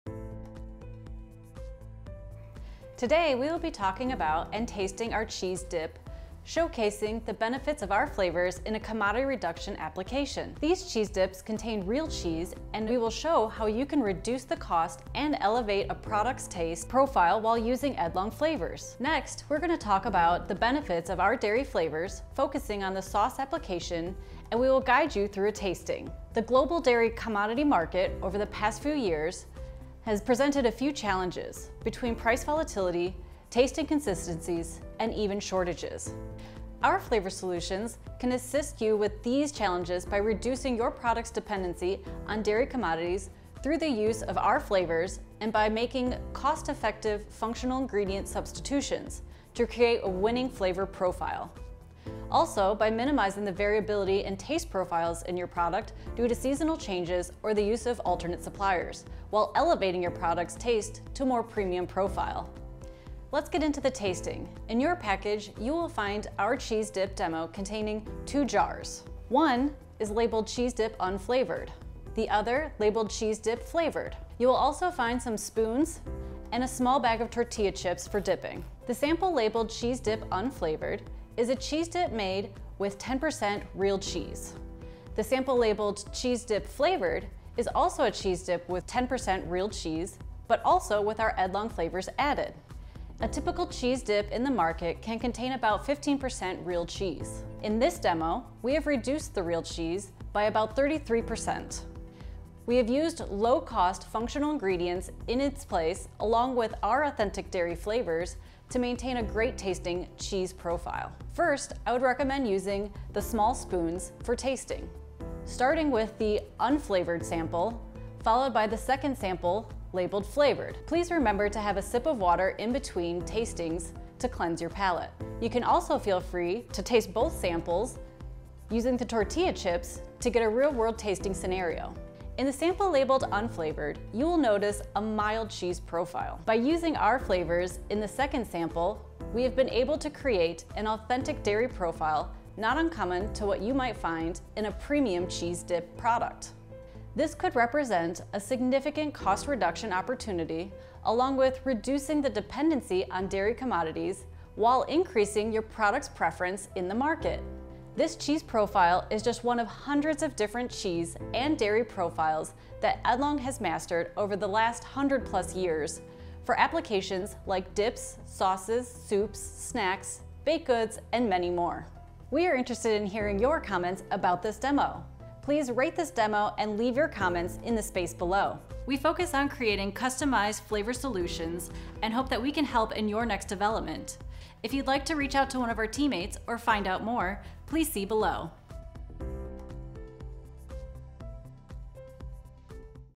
Edlong Cheddar Cheese Sauce Demo
This demo highlights our cheese flavors in a cheese sauce focusing on the profile enhancements that can be achieved with the use of Edlong Flavors.